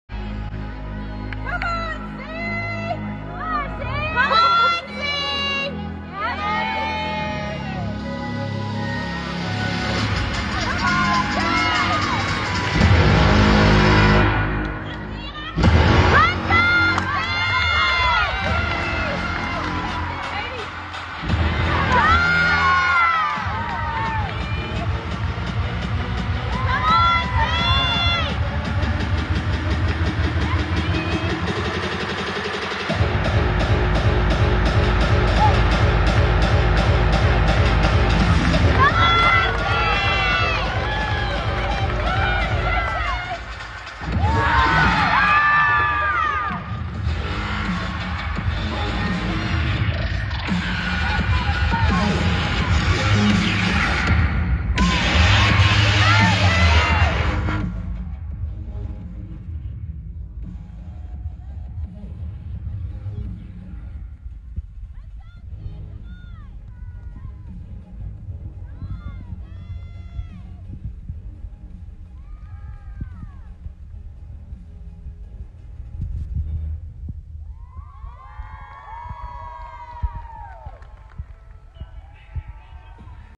Sienna Schreiber (Missouri) 2022 Intrasquad Floor
Intrasquad routine
12/04/2022
Video courtesy of Missouri Gymnastics.